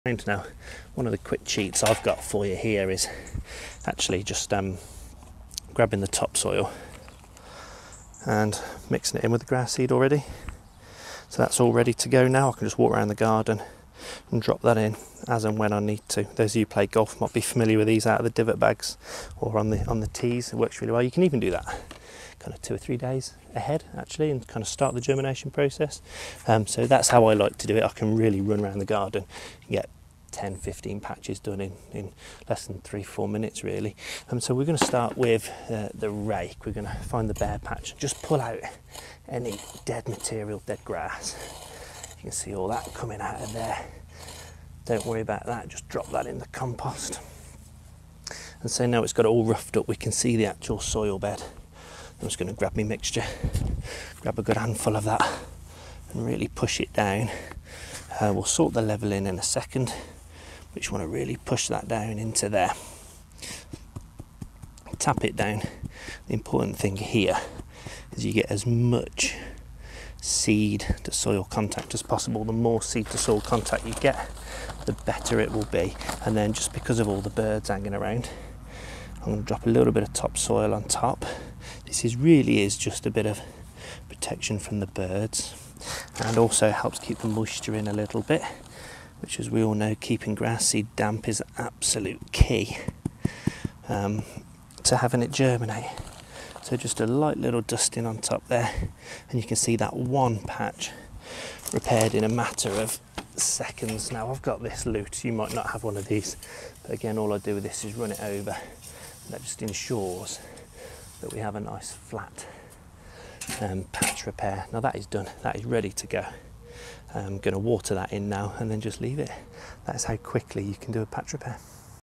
Lawn Care for Beginners - Repairing bare spots and dog patches in the lawn
This video looks at the quickest and best way to fill in those small bare patches in the lawn, weather caused by the kids or the dog.  Tried and tested methods from the golf course, straight to you.

It’s part of a longer more in-depth video series in which I have been repairing my lawn after workers dug a huge trench in my garden!  This includes lawn treatments such as fertilisers.